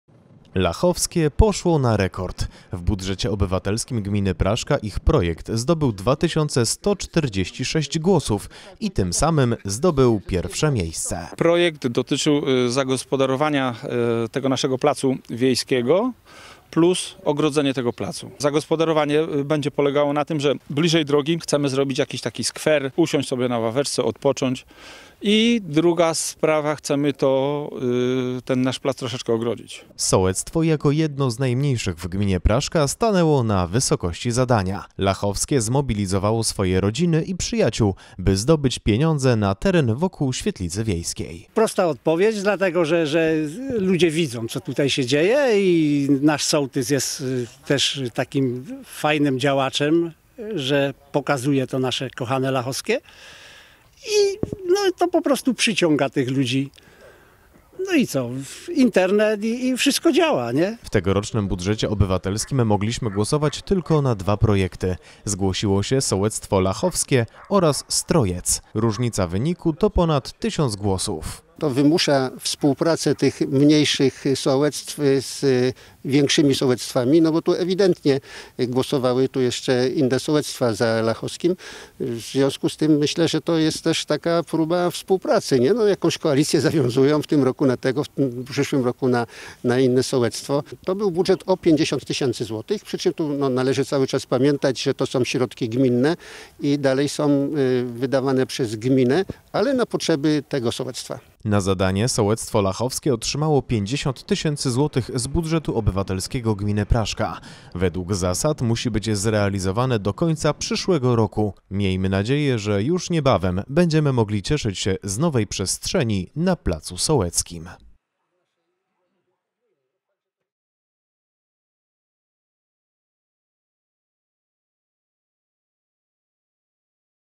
Lachowskie wygrało w Budżecie Obywatelskim! 2021
Lachowskie poszło na rekord. W Budżecie Obywatelskim Gminy Praszka ich projekt zdobył 2146 głosów i tym samym zdobył pierwsze miejsce.